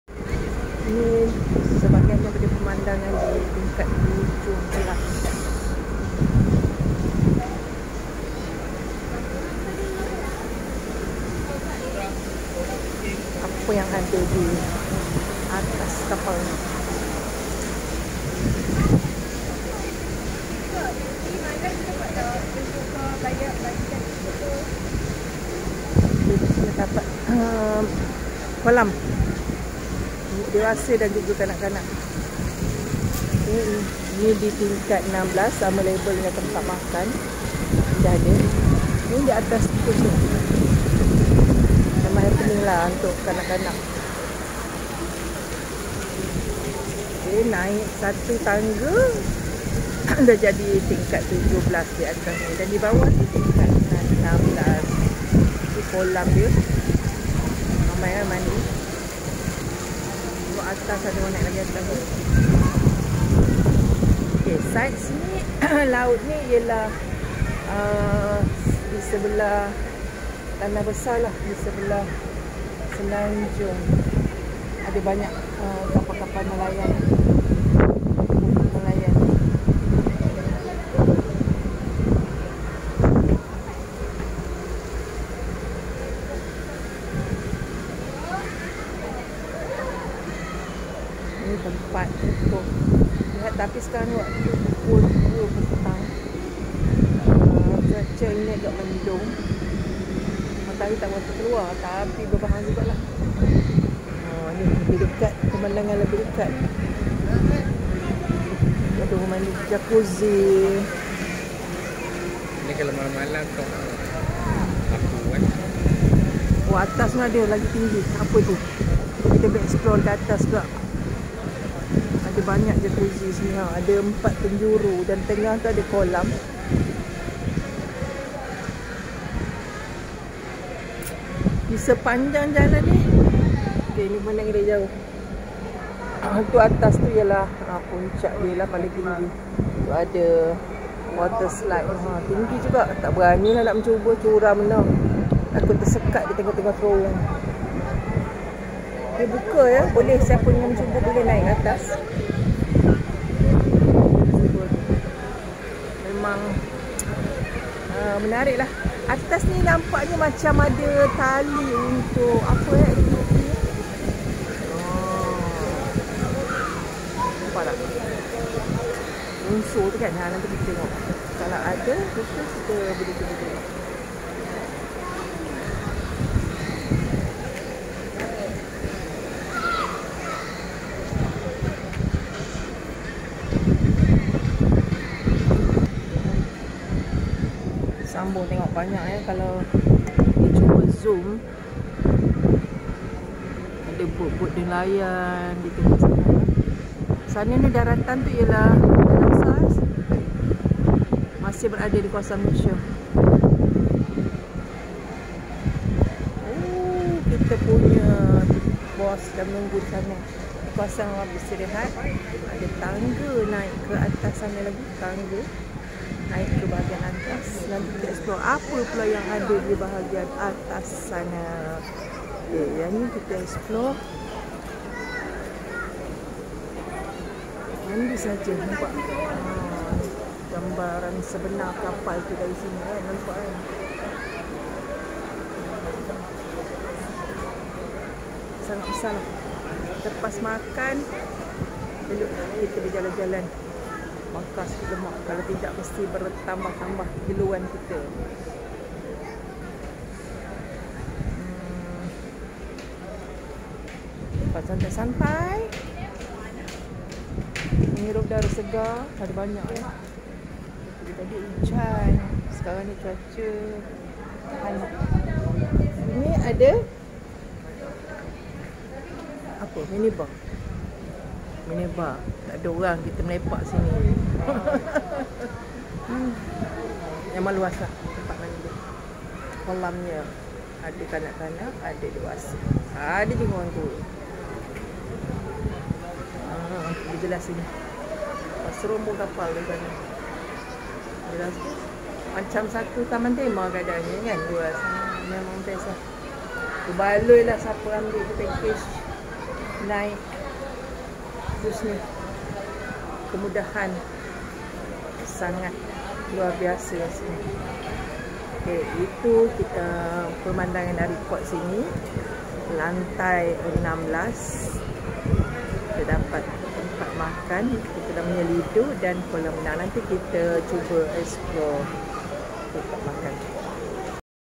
Resort World (Genting Cruise)
Pelayaran 4H3M tidak terasa, kerana kemudahan yg ada di atas cruise. Untuk video kali ini..kita explore tingkat 16 di atas cruise yg menempatkan kolam mandi,.jakuzi dan gelongsor air (water slide)

Tingkat 16 menjadi tumpuan utama kebanyakan org kita utk bersantai²...

Sangat menarik...